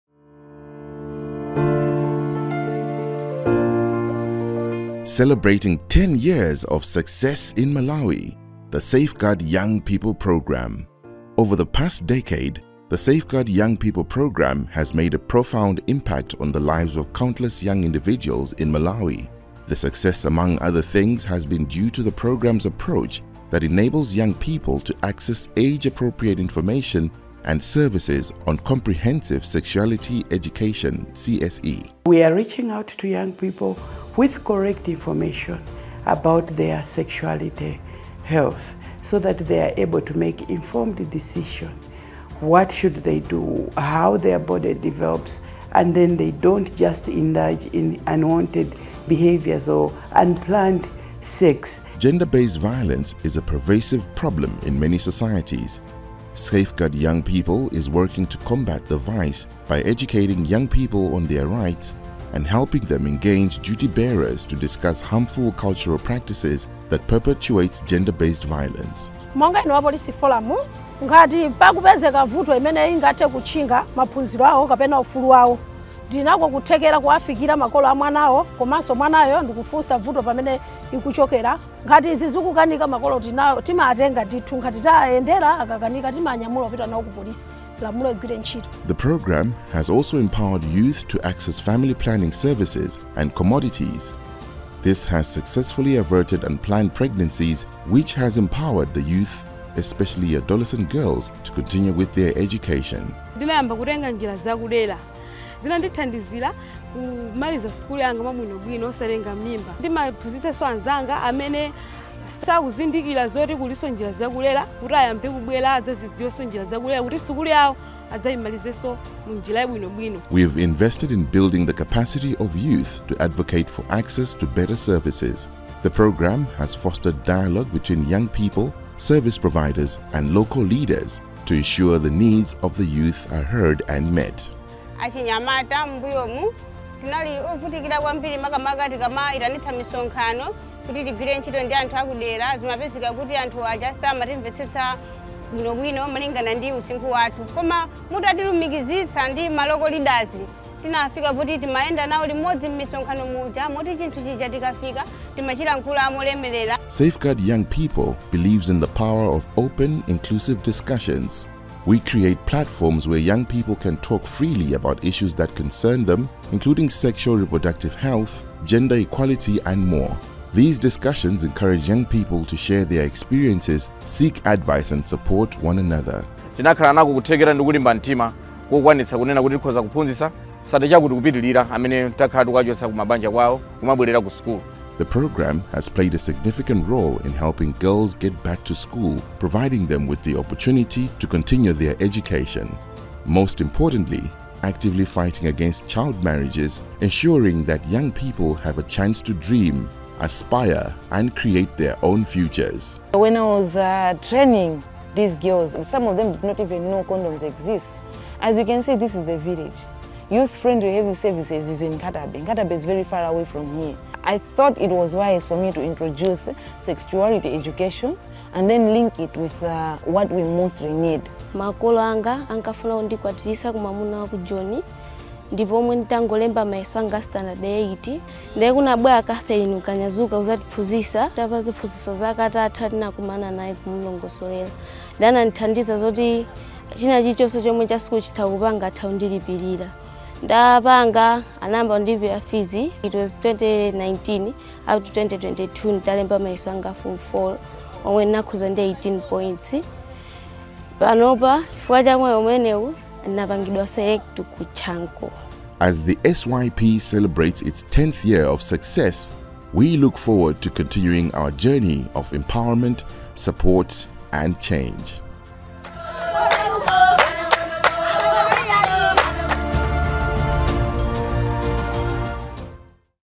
The Safeguard Young People Programme: Youth networks, activists and champions in Malawi
Over ten years, the Safeguard Young People Programme has played a pivotal role in nurturing a generation of vibrant youth leaders, unwavering advocates, and influential champions for adolescent sexual and reproductive health and rights. Through compelling stories and testimonials, we delve into the lives of young activists who have been at the forefront of advocating for meaningful policies and initiatives. Join us as we explore how the programme has catalyzed change, inspired action, and forged strong networks among young people across regions. Witness the power of youth advocacy in action, and see how together, we are building a brighter future for all.